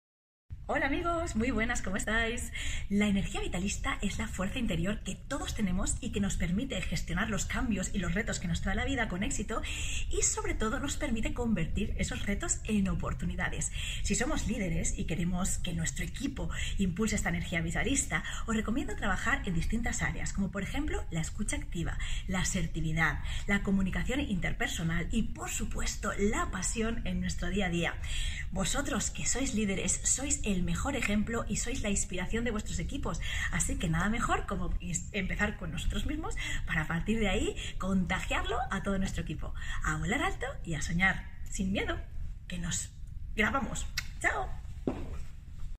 Ami Bondía - Lidera con Energía Vitalista
🚀Ami Bondía: “Como líder, tu entusiasmo y visión pueden convertir cualquier desafío en una oportunidad para que tu equipo sea más productivo”.

Para liderar, es importante conectar emocionalmente, inspirar con visión y demostrar pasión en cada acción. Crear un entorno donde cada miembro se sienta valorado y empoderado para dar lo mejor de sí. Cuando un líder promueve la energía vitalista, no solo se alcanzan metas, ¡se superan!

🚀💪 Deja que tu liderazgo ilumine el camino y desata el potencial ilimitado de tu equipo. ¡La verdadera magia ocurre cuando todos se sienten inspirados y comprometidos! 🌟